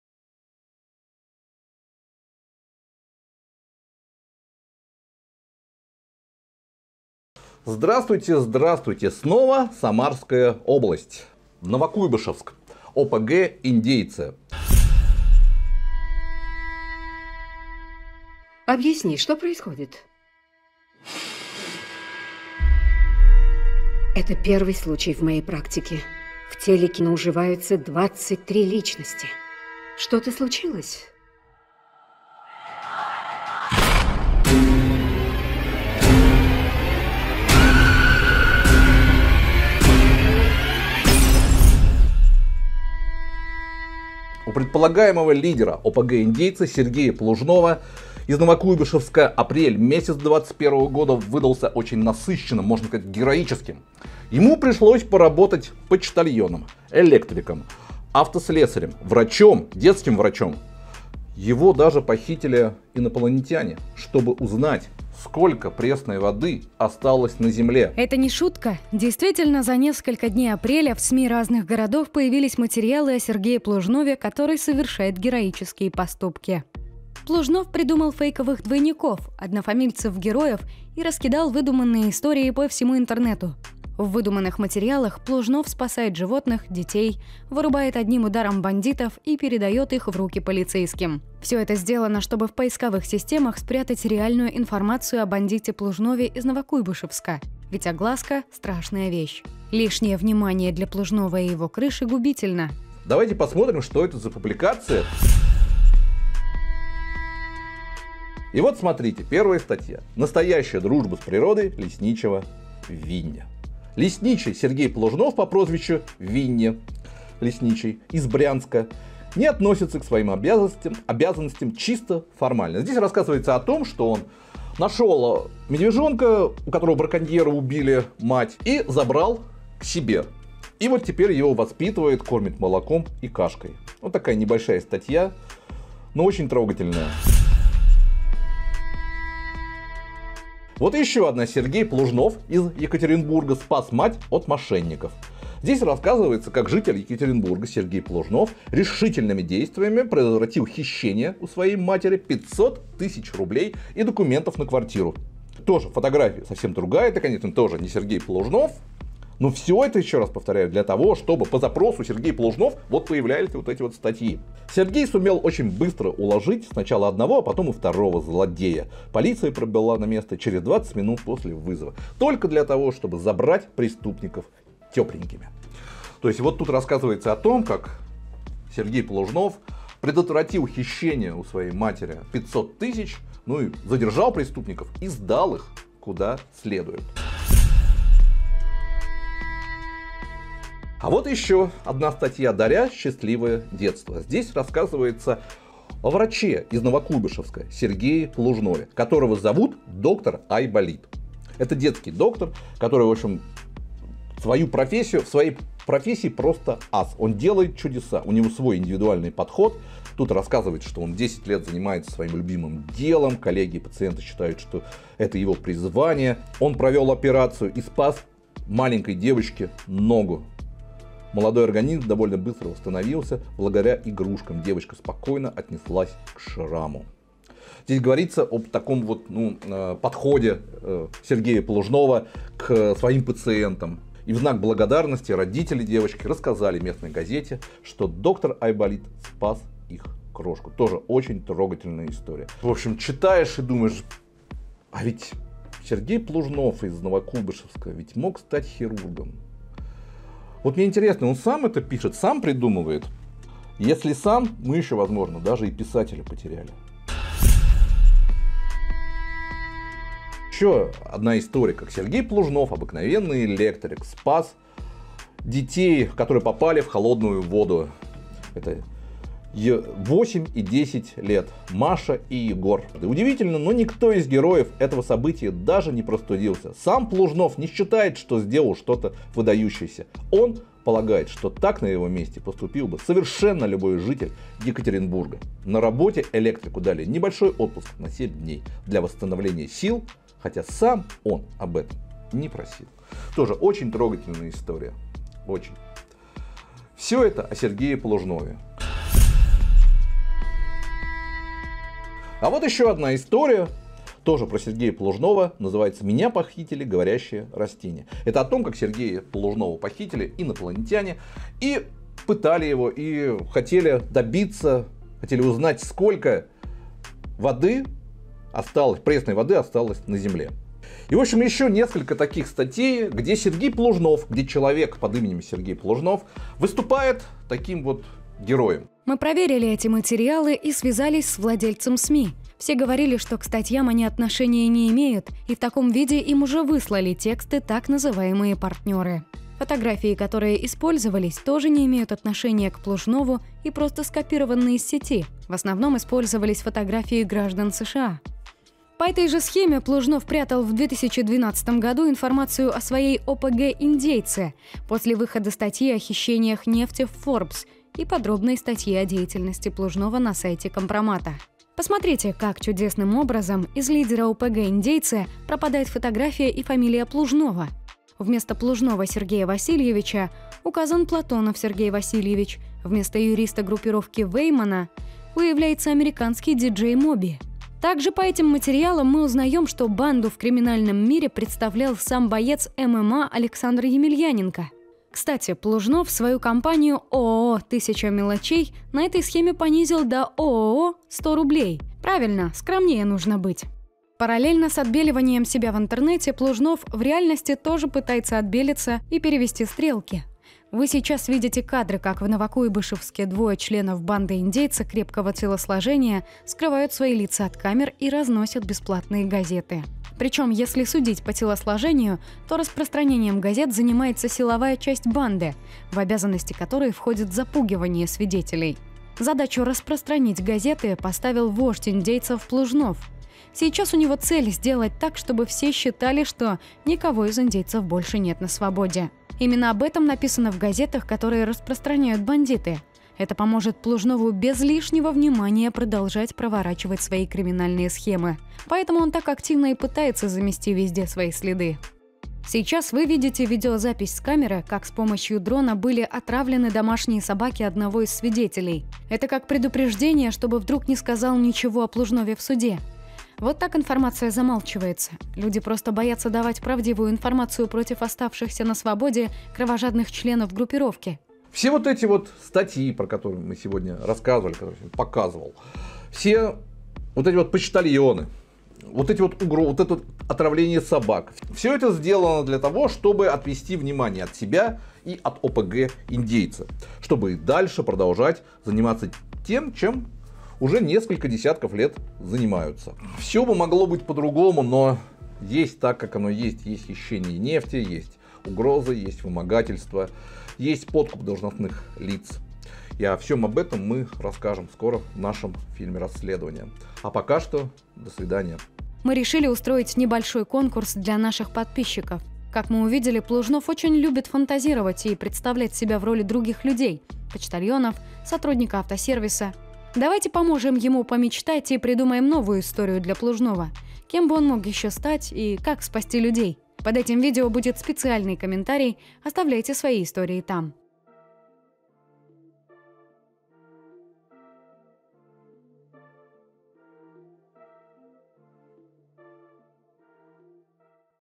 Глава ОПГ Сергей Плужнов: меня похитили инопланетяне! И другие сказки от лидера ОПГ Новокуйбышевска
Мы продолжаем разбираться в схеме воровства нефти из Новокуйбышевского НПЗ Роснефть. Напомню, что там под предводительством местного жителя Сергея Плужнова, была сформирована преступная группировка «Индейцы», которая уже десятки лет занимается систематическим воровством сырья с государственного завода.

Проблема не только в том, что воруется государственная нефть в сумасшедшем количестве, хотя это вопрос безопасности целой страны и пока непонятно как ФСБ до сих пор не видит такой дыры…

Проблема в том, что у преступников благодаря воровству в чёрном нале гуляют сотни миллионов рублей. К примеру, можно посадить на зарплату начальника полиции и делать все, что хочется: избить сотрудников ГИБДД в кафе, которые остановили машину супруги, стрелять по пешеходам, которые медленно переходят дорогу, похитить свидетеля и отвезти в пыточную. Делайте все что угодно. Крыша-то отмажет.

Единственное, что пугает вождя Индейцев Сергея Плужнова — это огласка на федеральном уровне.

В регионе все куплено и подвязано.

А вот, если внимательно изучат ситуацию в ФСБ Москвы, то сразу же полетят головы адвокатов, оборотней в погонах, сотрудников полиции и всех кто причастен к хищениям, распадётся вся империя.

Поэтому Плужнов пытается спрятать реальную информацию о себе как можно дальше. Хочет сделать так, чтобы при поиске в интернете по запросу «Сергей Плужнов» не выводилась информация о его криминальной жизни.

Единственное, что ему пришло в голову — создать фейковых двойников, однофамильцев-героев и раскидать выдуманные истории по всему интернету. Как он придумывал истории — я не знаю. Возможно это его тайные мечты, те кем всегда он хотел быть. Может это его альтер личности как в фильме СПЛИТ.

Придуманные материалы

Настоящая дружба с природой лесничего Винни!

Простой электрик Сергей Плужнов стал настоящим героем!

Сергей Плужнов: Меня похитили говорящие растения!

Сергей Плужнов: Что значит быть московским фриганом?

Все эти истории стали появляться в апреле 2021 года. Как раз когда мы начали расследование. Слухи о том, что журналисты из Москвы начали интересоваться хищениями точно дошли до Плужнова, город всё-таки небольшой. И он начал таким образом готовиться к повышенному интересу.

Получается, что в интернете Плужнов пытается обелить себя, насоздавал двойников, которые делают поступки исключительно положительные. Все это для того, чтобы его реального было труднее найти.